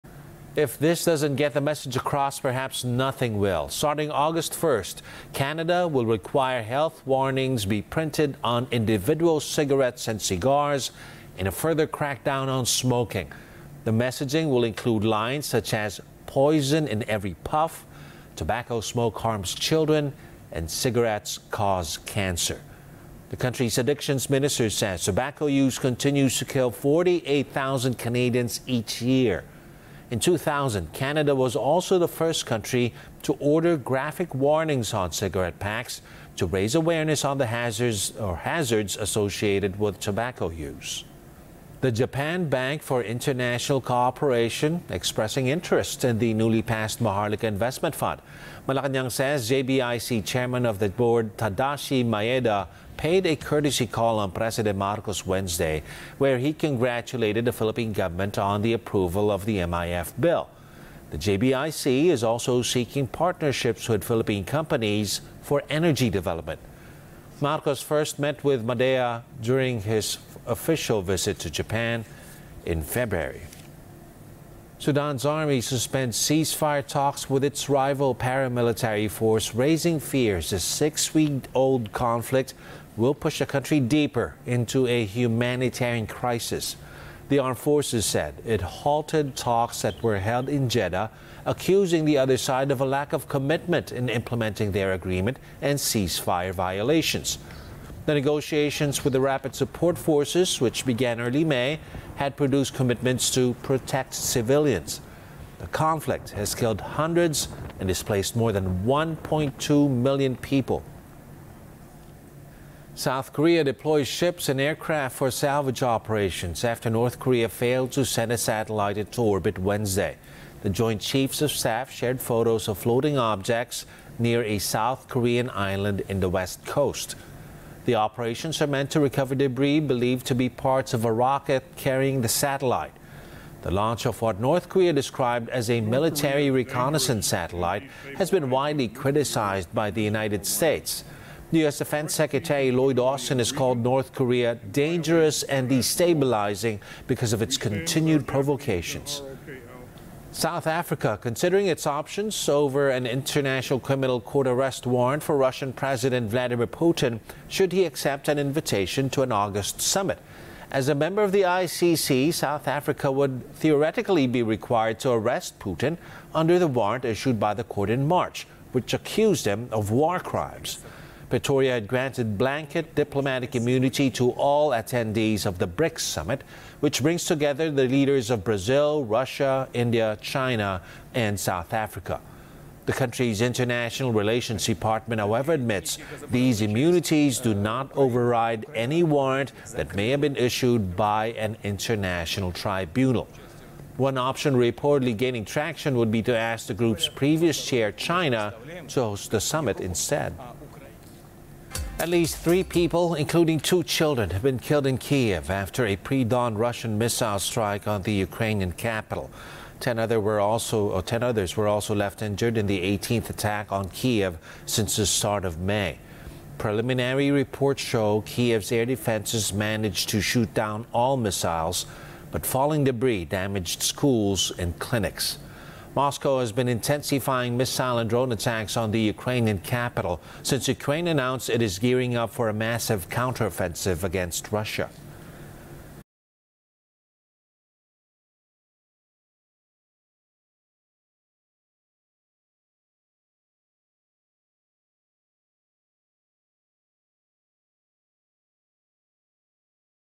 Canada to require health warnings on individual cigarettes | ANC
Rundown: Starting August 1st, Canada will require health warnings to be printed on individual cigarettes and cigars in a further crackdown on smoking.